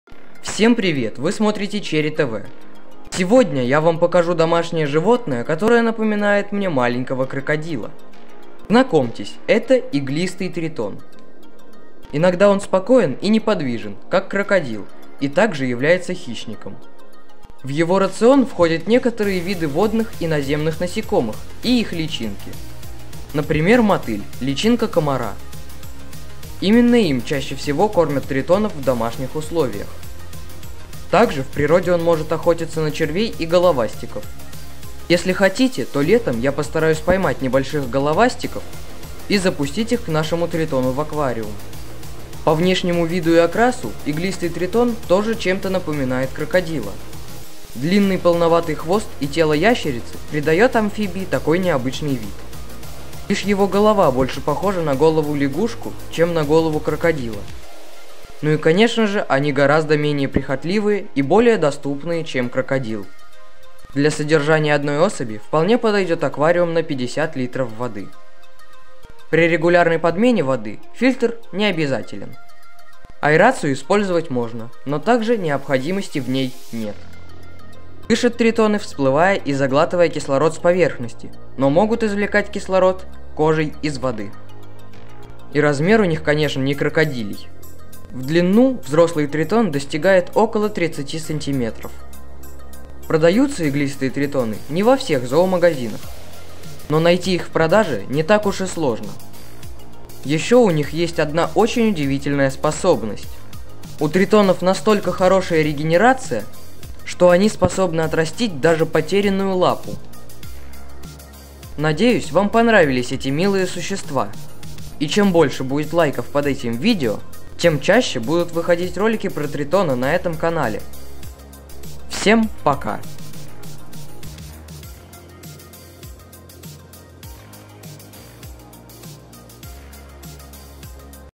Мини-Крокодил в домашних условиях! | Содержание иглистого тритона!
В этом видео я покажу как ухаживать за иглистым тритоном!